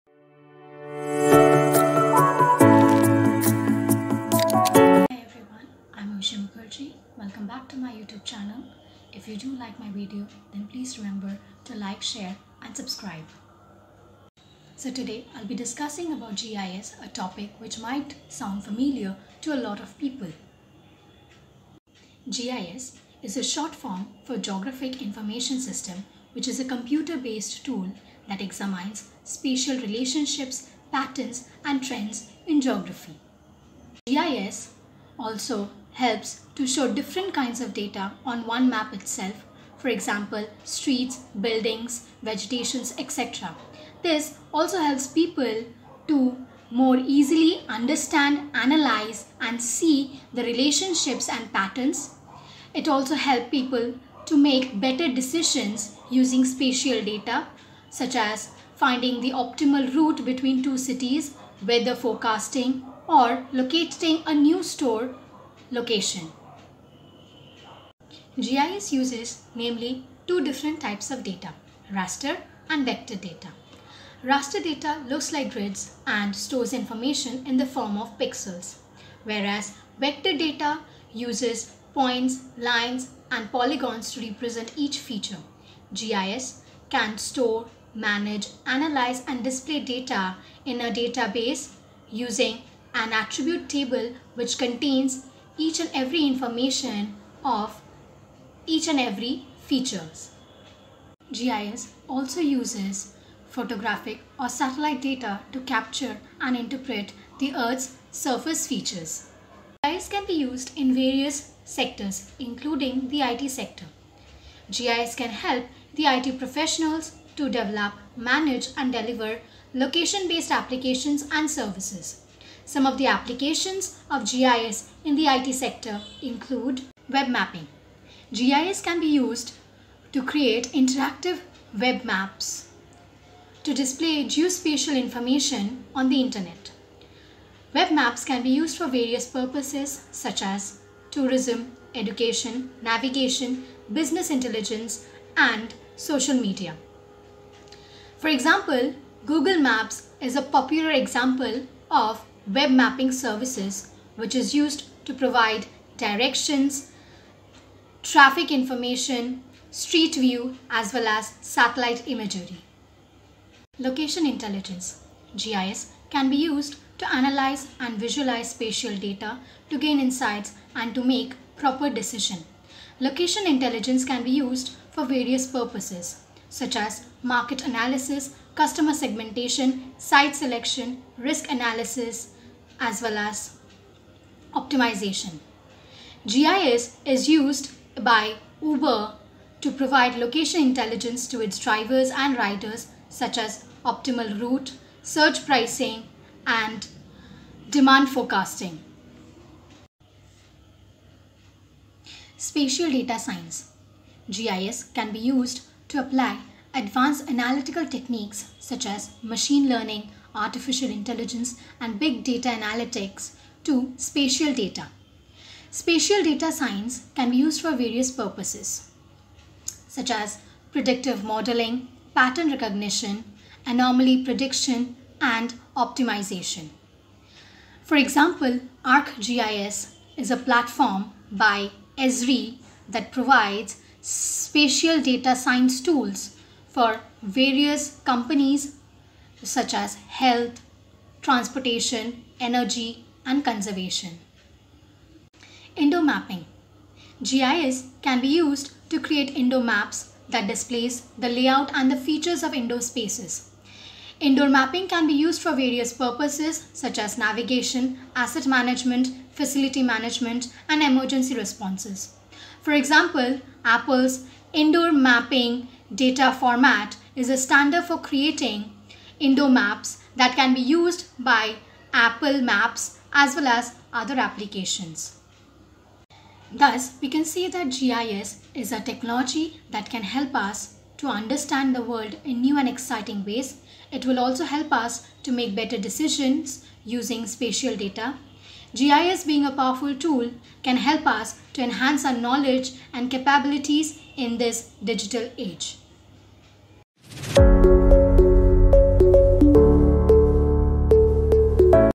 Geographical Information System(GIS)
GIS is more than just a technology. It is a way of thinking spatially and using geography to understand the world. With GIS, you can discover new insights, explore new possibilities, and make a difference in your community and beyond.